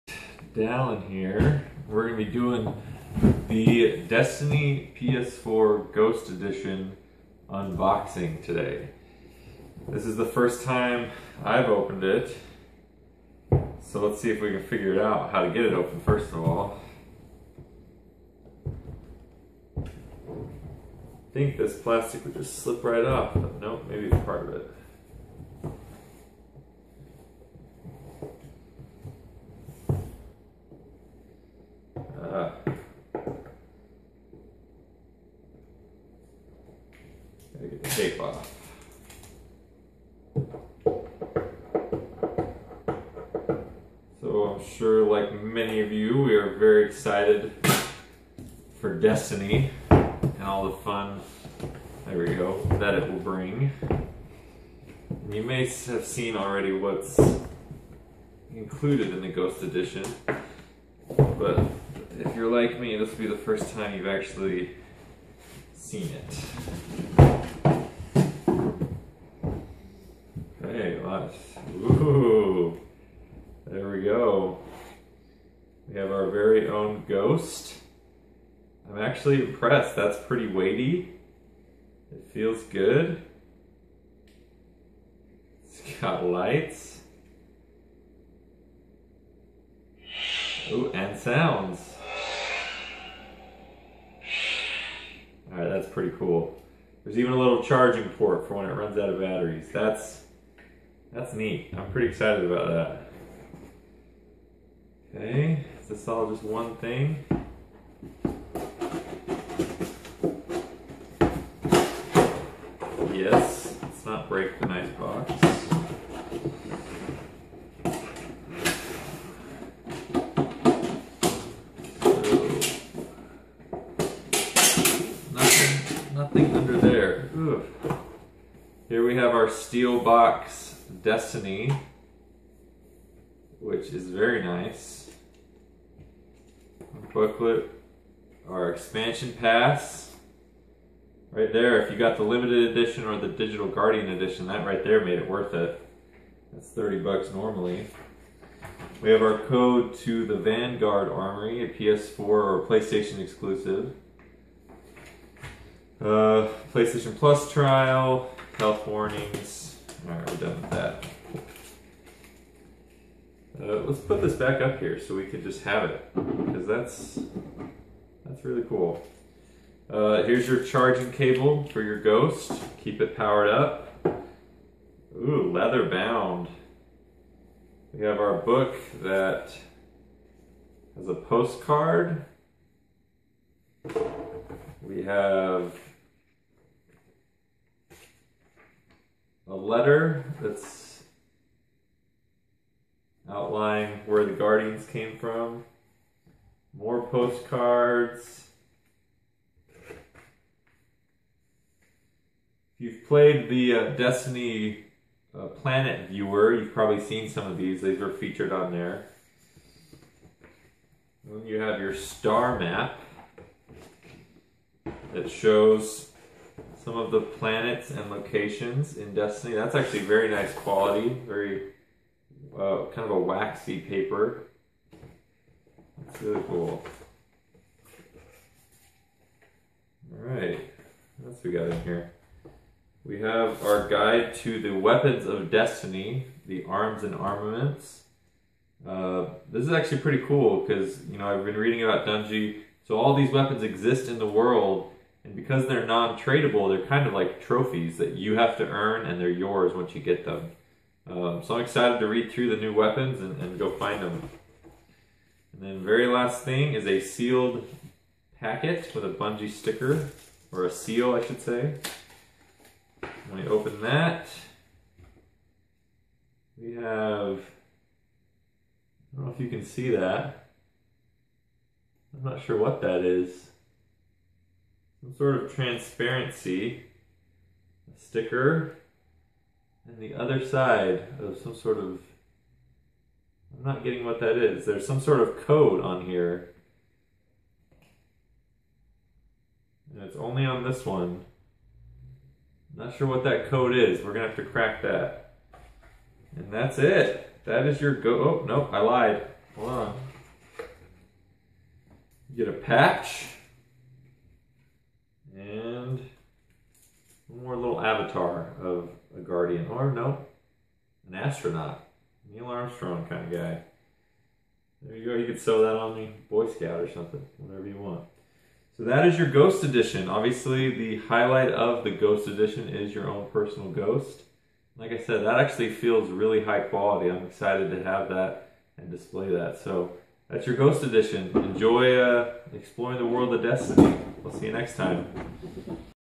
Destiny Ghost Edition UNBOXING
This is the PS4 edition so it has some small extra goodies, but it is essentially the same for all platforms.